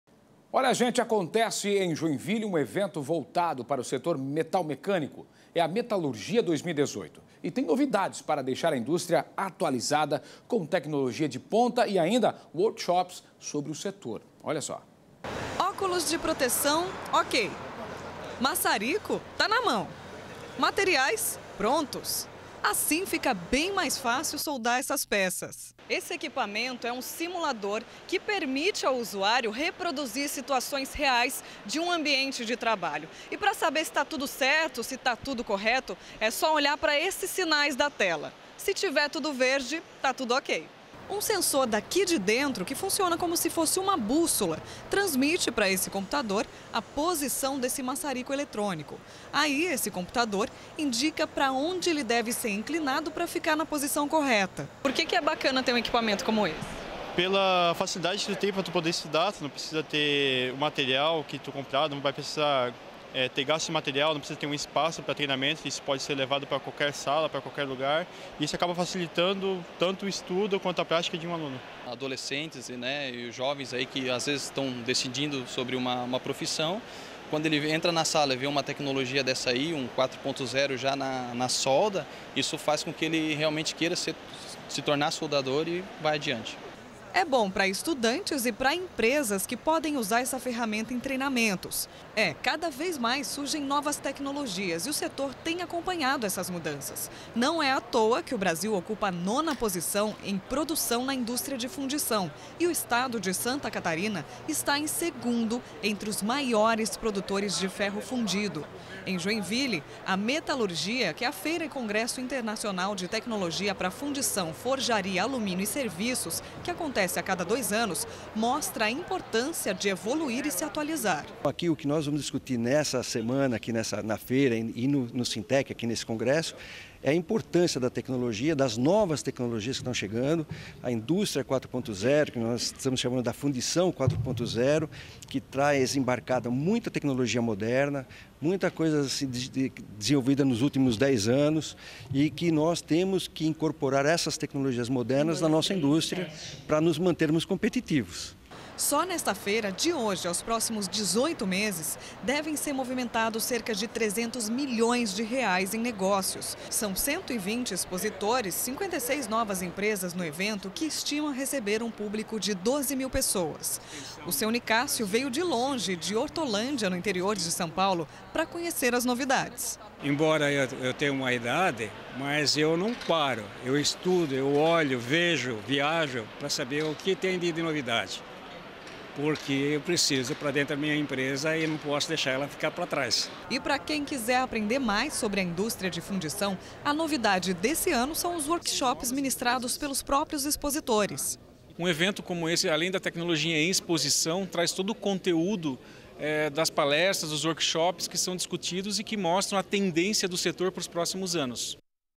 Metalurgia 2018: novidades para indústria metal mecânica e de fundição
Acontece em Joinville um evento voltado para o setor metal mecânico, a Metalurgia 2018. Têm novidades para deixar a indústria atualizada com tecnologia de ponta e ainda workshops sobre o setor.